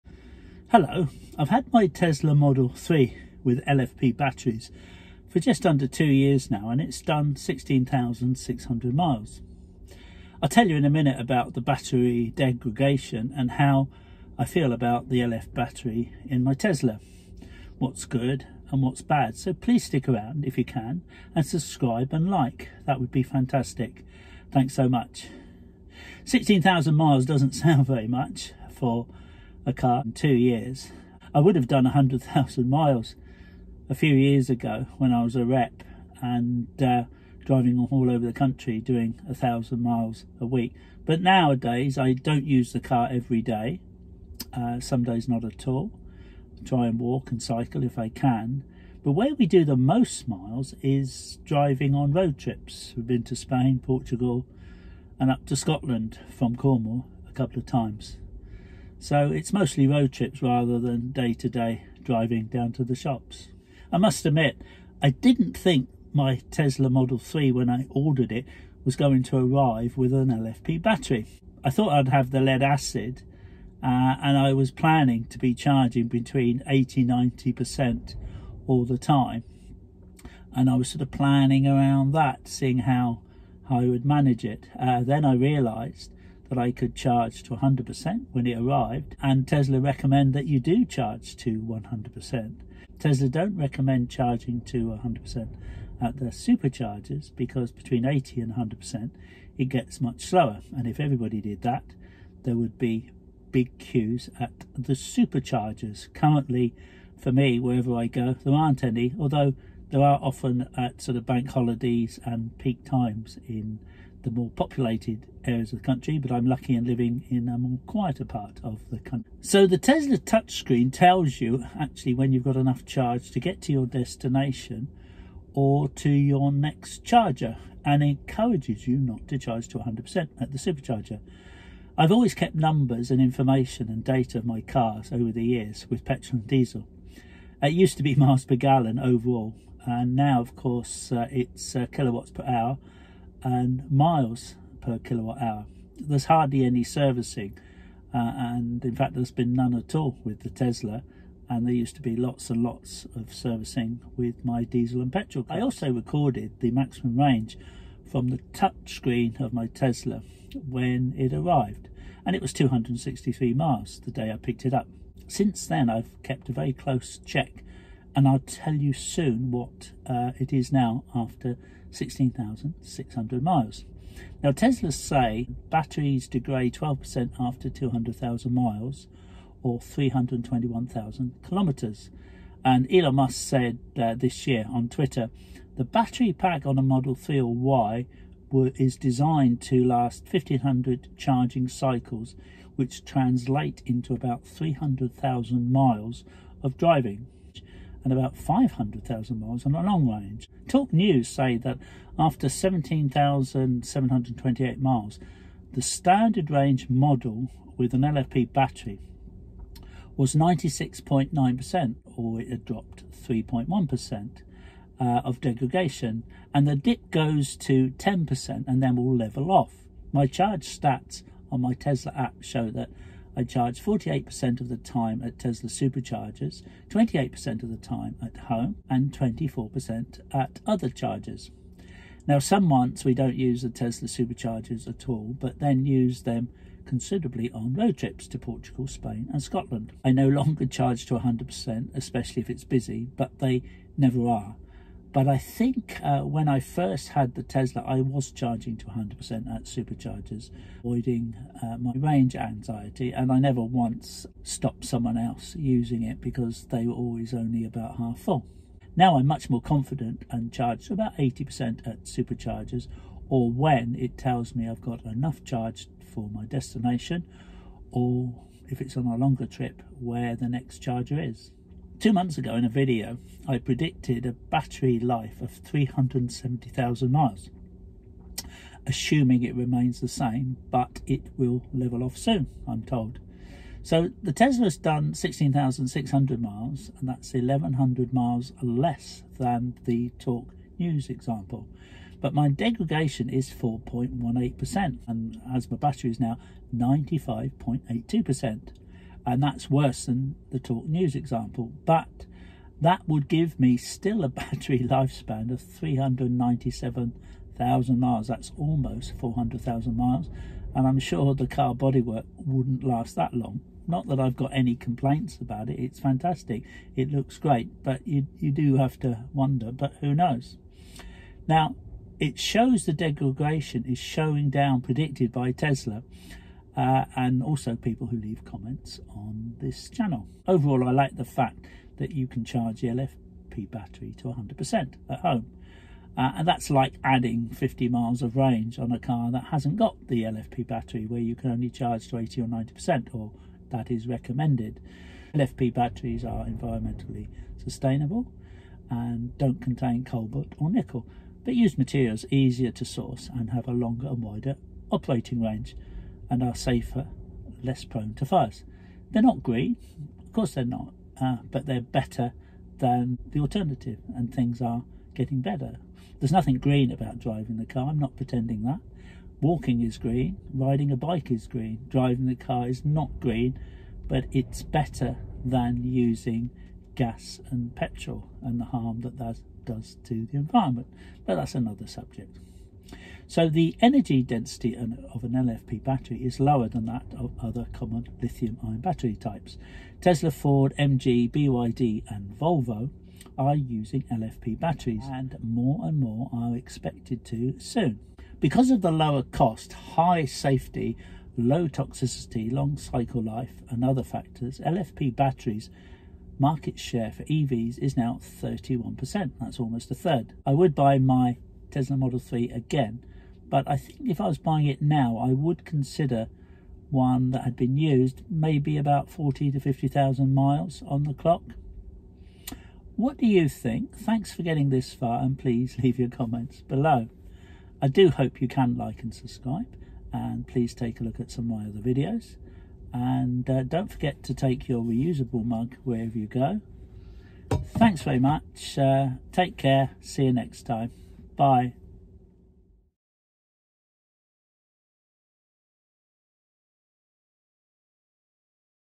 Tesla LFP Battery
Living with a Tesla, LFP, battery model 3  and charging to 100% with the degradation figures after almost 2 years